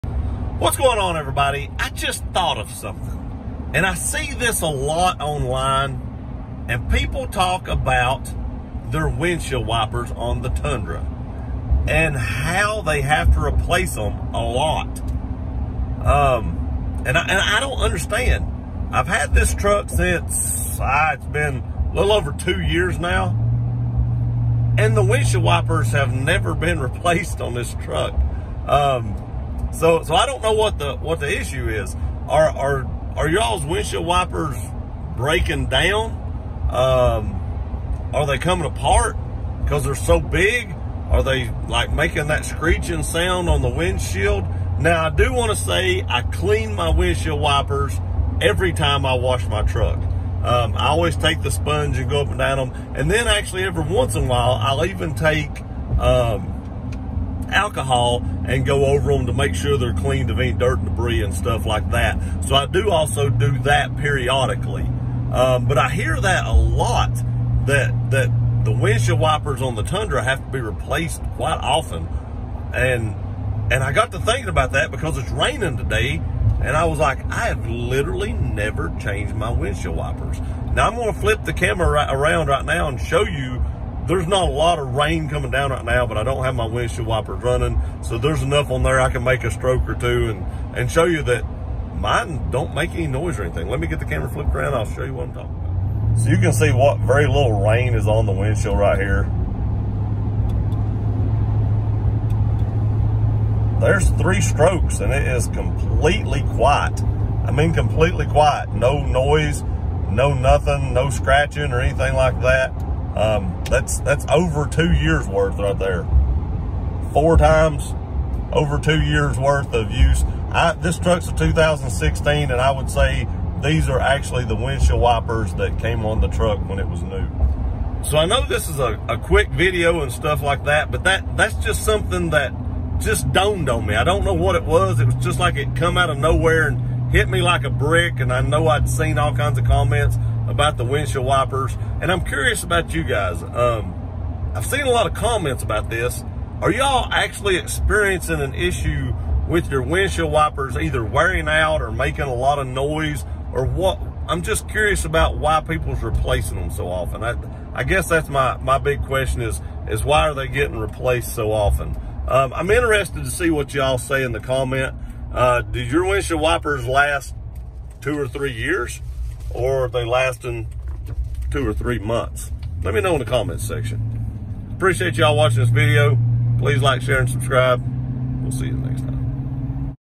Do You Have This Problem With Your Tundra
I’ve seen this comment several times and was wondering how many of you all have this problem with your Tundra?  Please leave a comment letting me know.